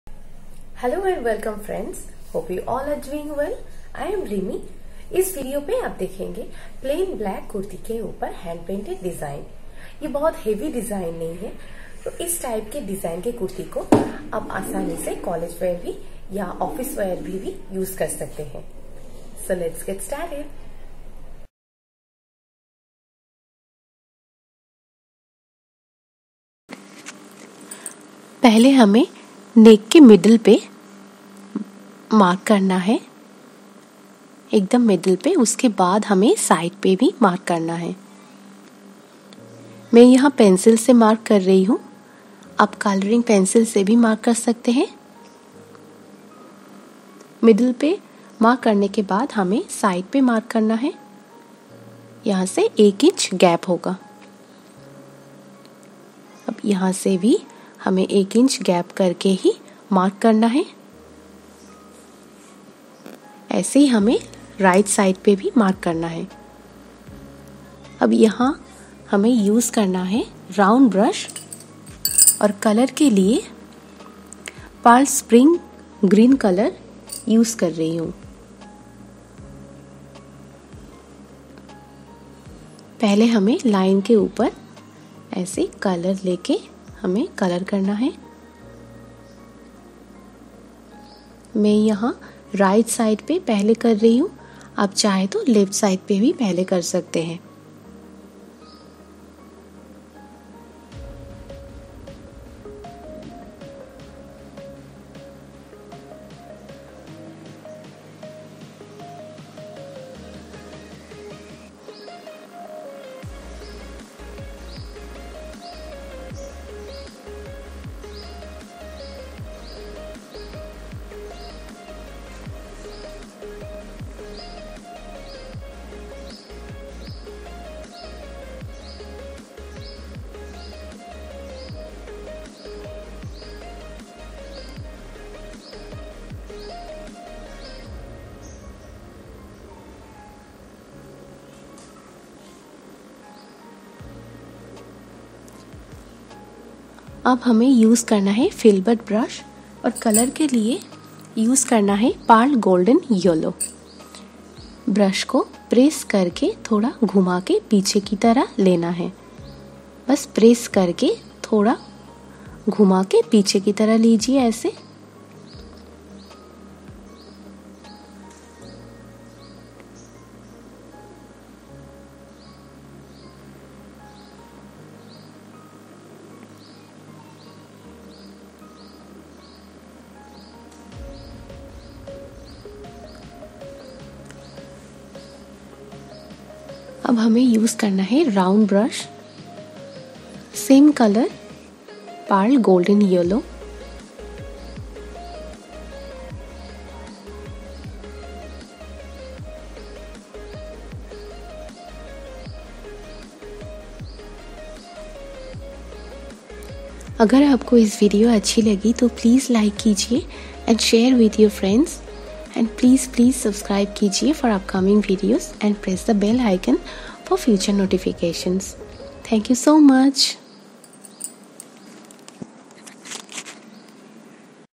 Super Stylish Designer Neckline on black colour SUIT/KURTI || Office & College wear for Girls.
In this video I have painted on a simple black kurti. This type of design can be wear in Office, College.

Rimi